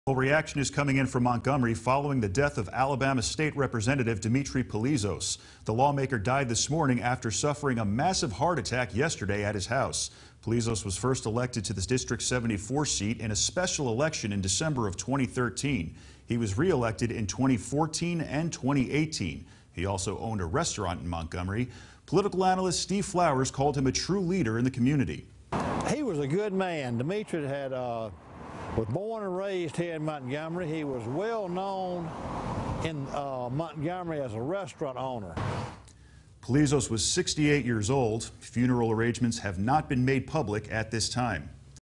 Mourning State Rep. Dimitri Polizos
State Representative Dimitri Polizos passed away yesterday and is being remembered by his fellow lawmakers.